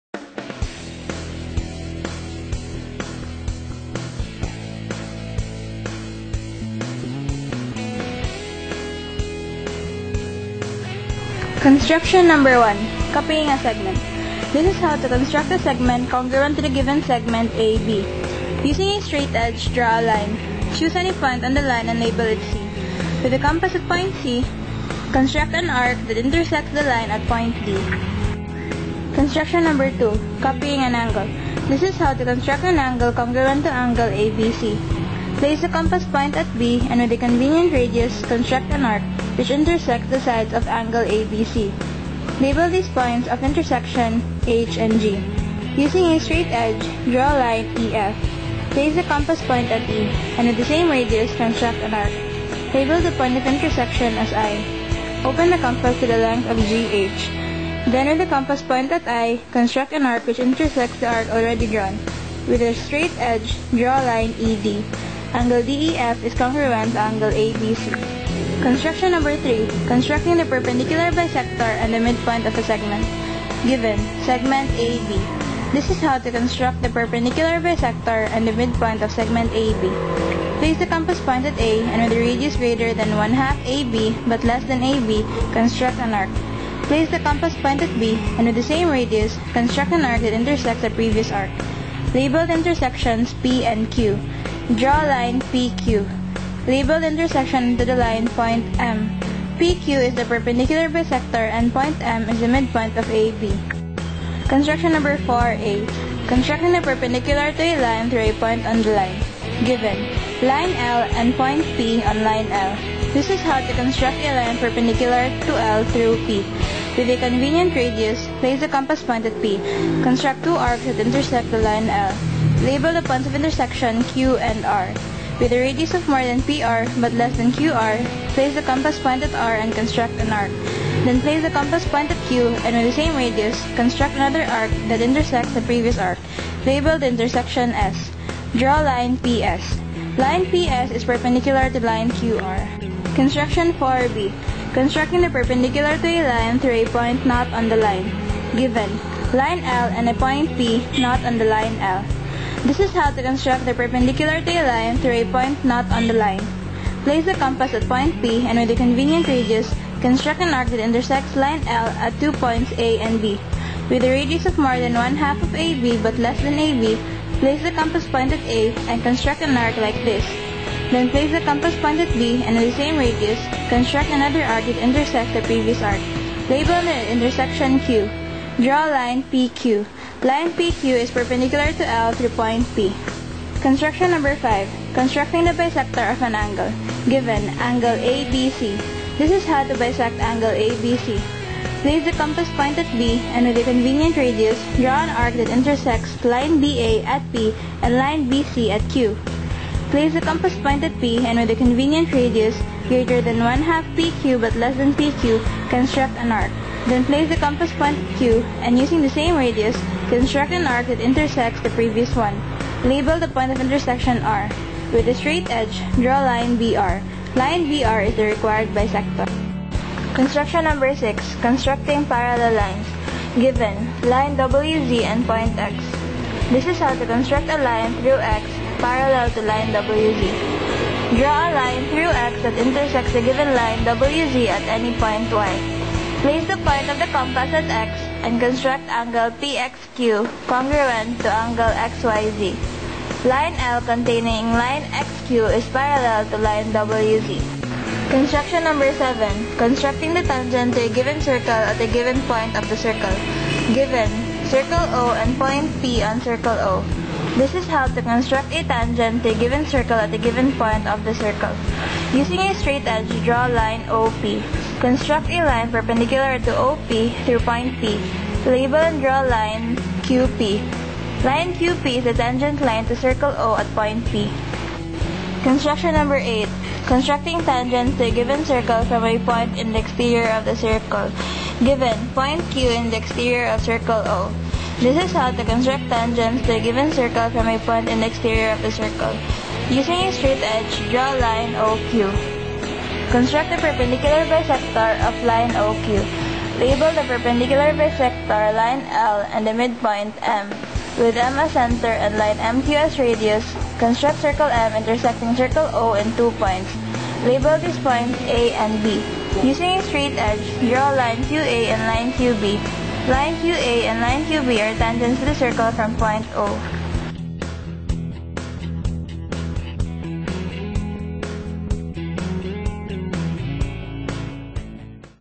Geometric Construction
This is a tutorial about a Geometry topic called Geometric Constructions. It serves as my 1st quarter project for math-iii. :D